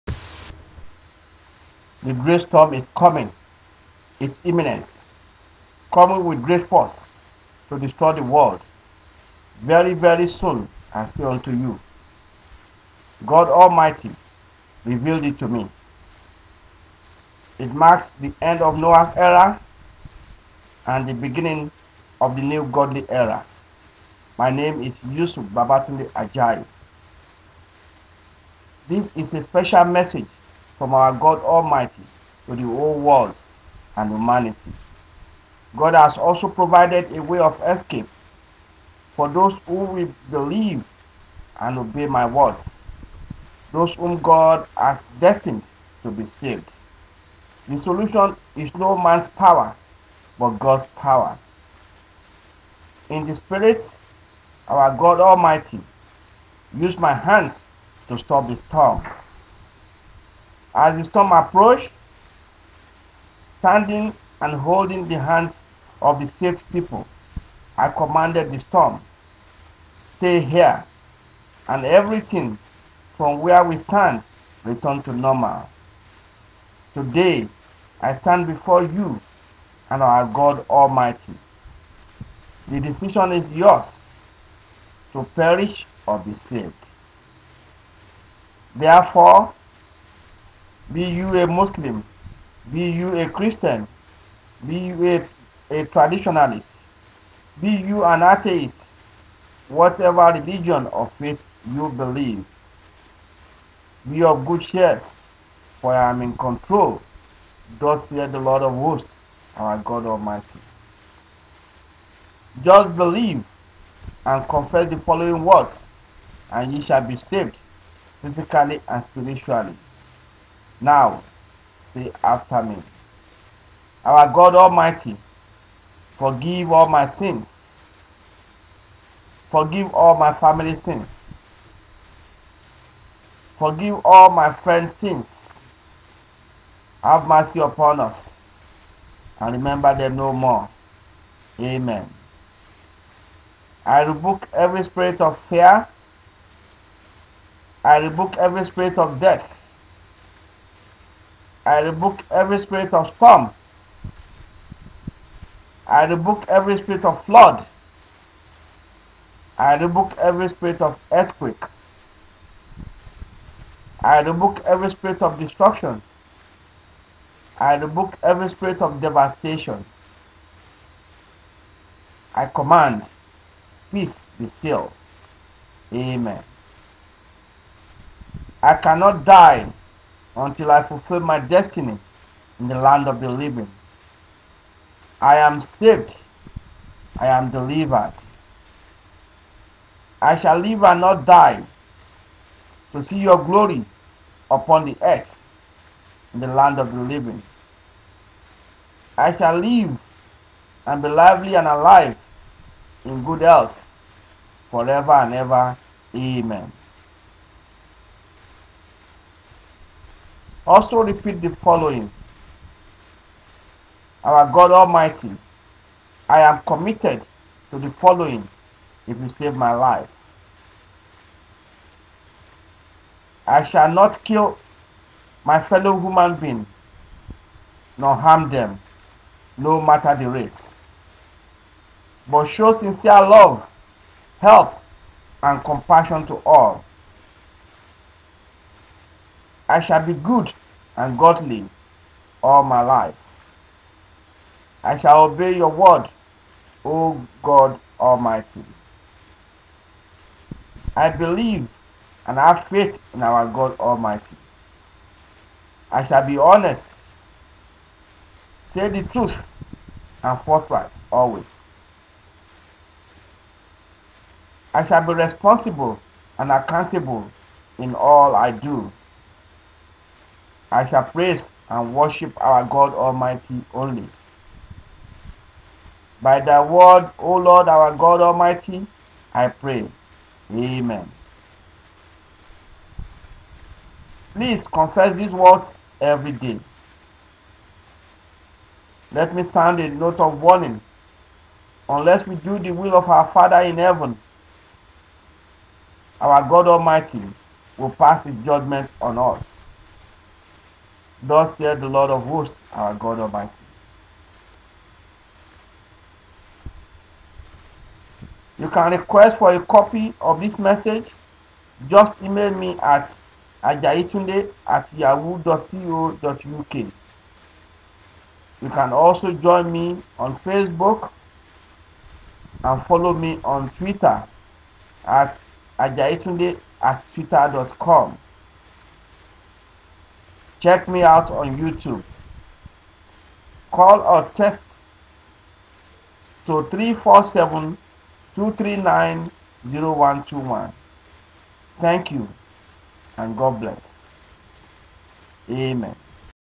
THE GREAT STORM (PERISH OR BE SAVED)
THE GREAT STORM (PERISH OR BE SAVED) IS REVEALED BY OUR GOD ALMIGHTY TO YUSUF BABATUNDE AJAYI ABOUT THE COMING DESTRUCTION OF THE WORLD BY STORM AND GOD ALSO PROVIDED OPPORTUNITY FOR PEOPLE WHO CONFESSED THE WORD TO BE SAVED AND SURVIVE TO THE NEW GODLY ERA